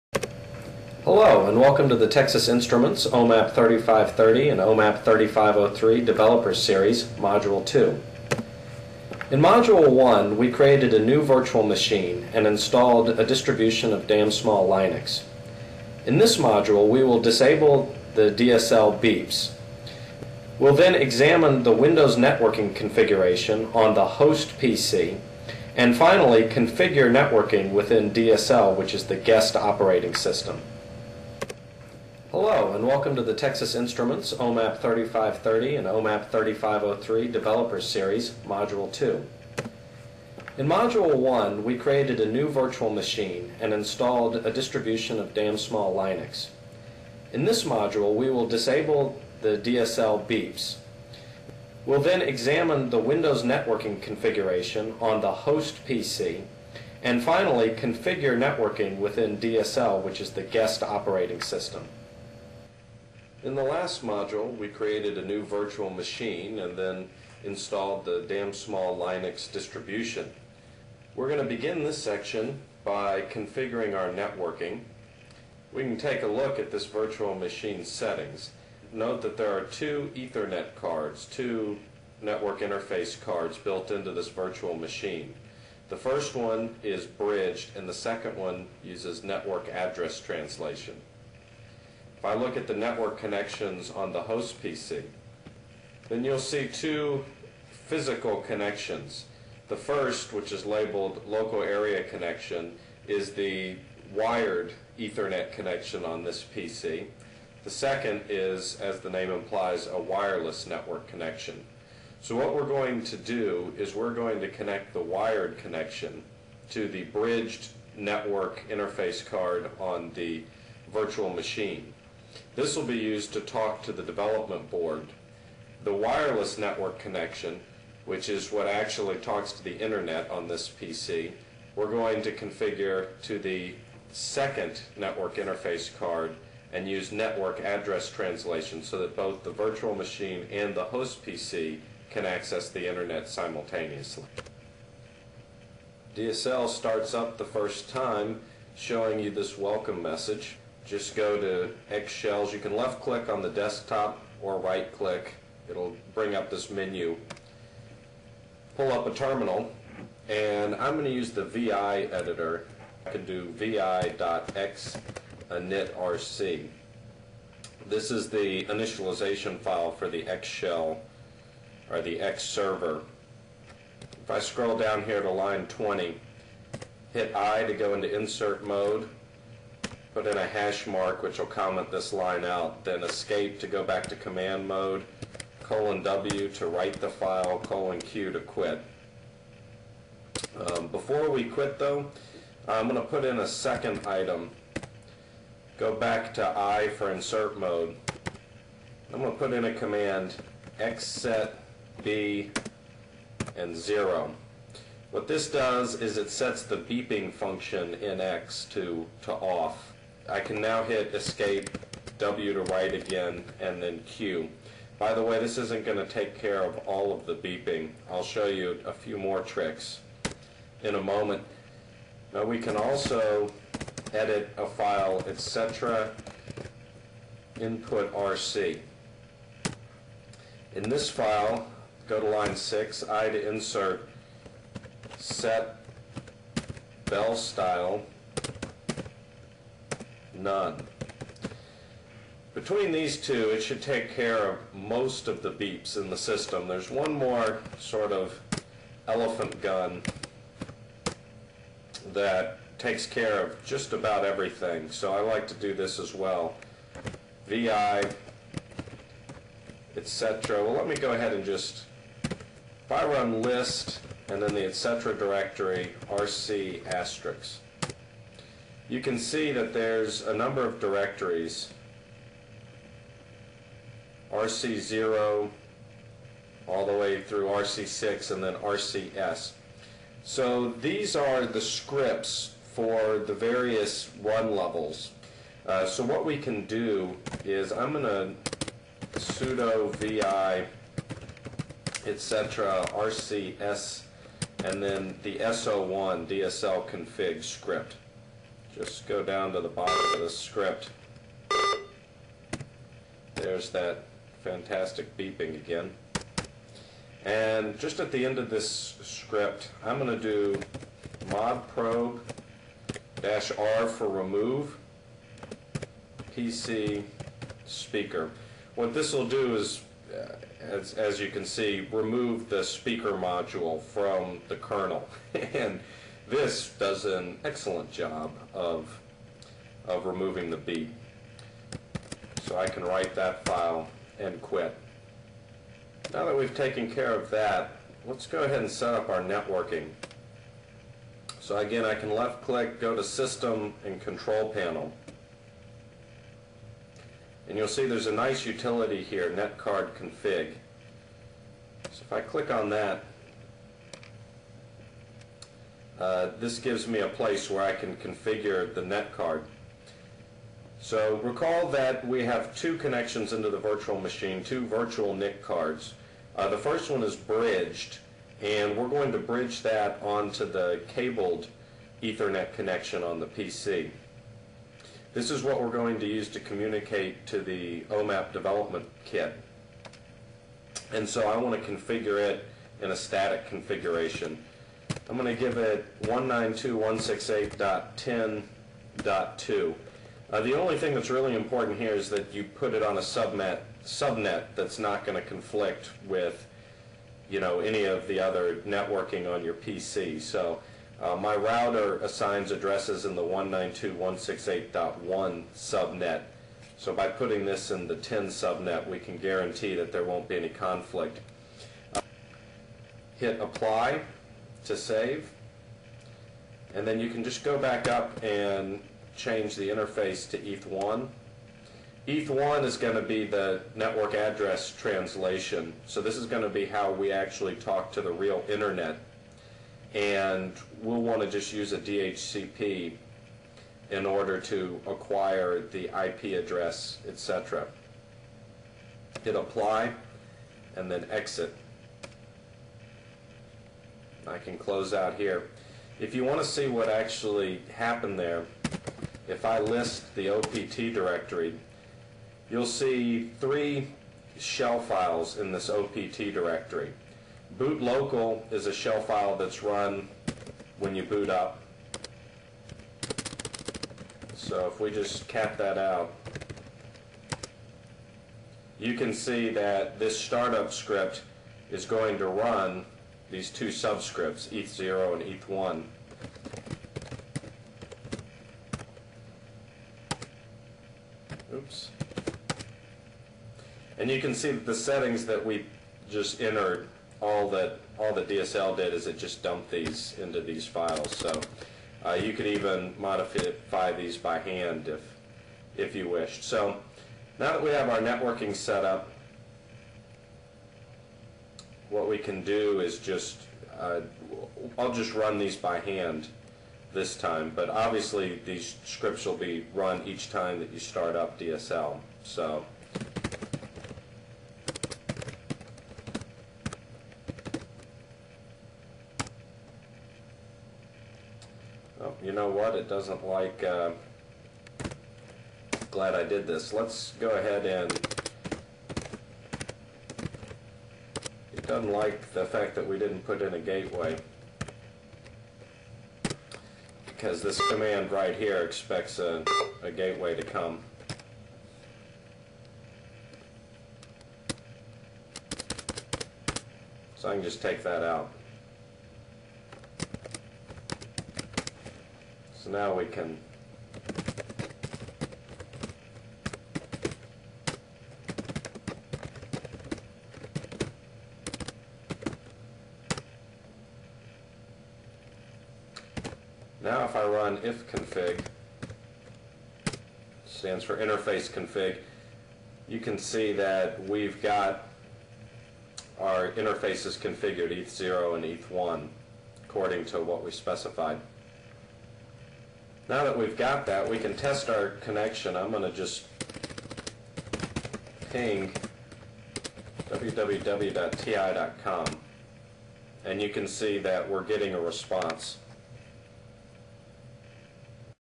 OMAP Developer's Series, Module 2
This online series will help you to quickly set up a development environment for the Texas Instruments OMAP3503 and OMAP3530 processors. In this second module, we configure the networking interfaces for the Damn Small Linux virtual machine created in module 1.